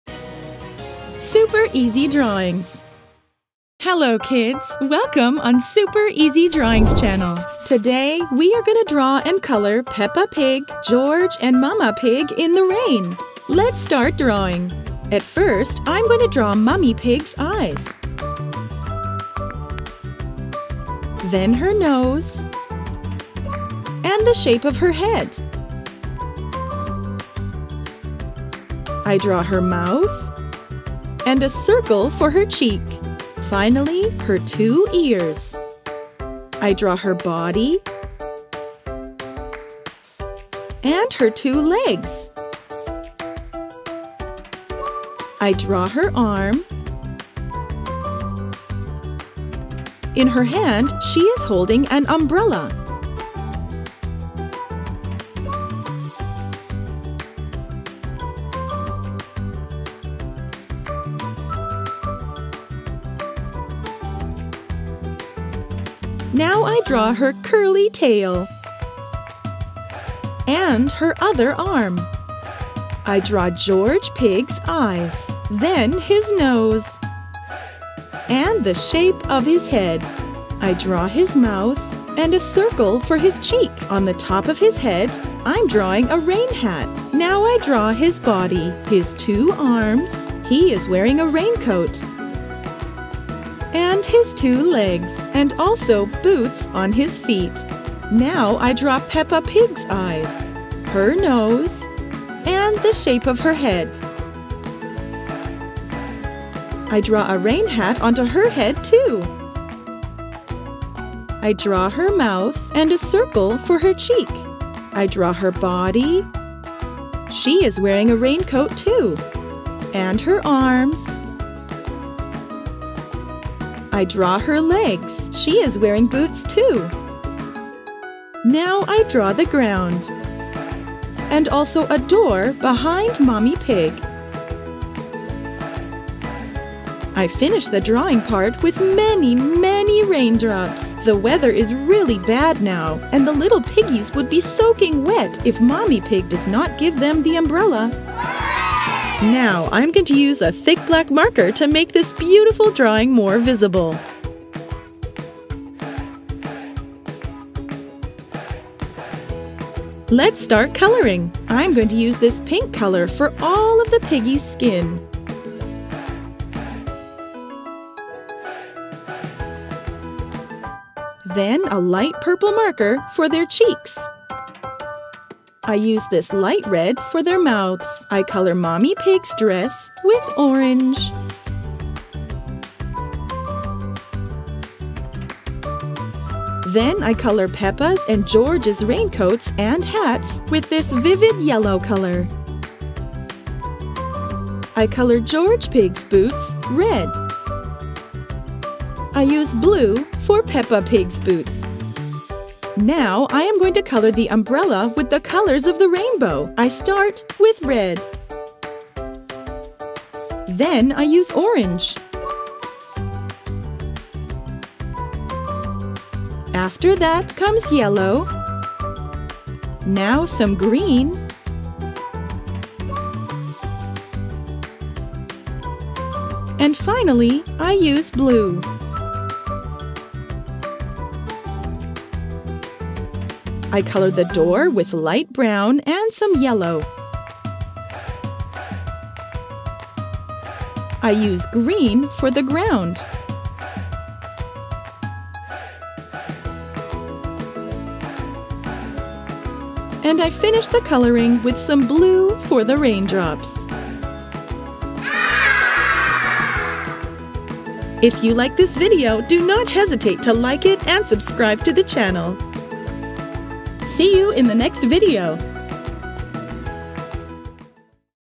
Drawing And Coloring Peppa Pig, George Pig And Mommy Pig In The Rain 🐷☔🌧️🌈 Drawings For Kids
In This Video I Show You How To Draw Peppa Pig, George Pig And Mommy Pig In The Rain. Drawing Tutorial for Peppa Pig!

This Video Is About How To Draw A Peppa Pig, George Pig And Mommy Pig In The Rain Step By Step: